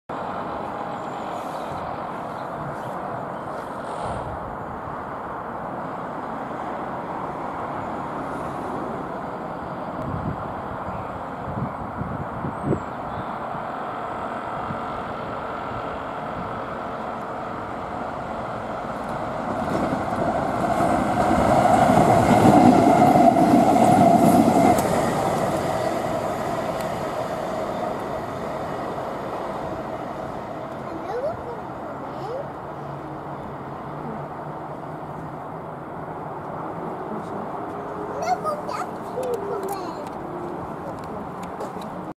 70 x 3
Saturday 28th December 2019 saw 70's numbers 70808, 70802 and 70806 head through Chesterfield from Westbury bound for Doncaster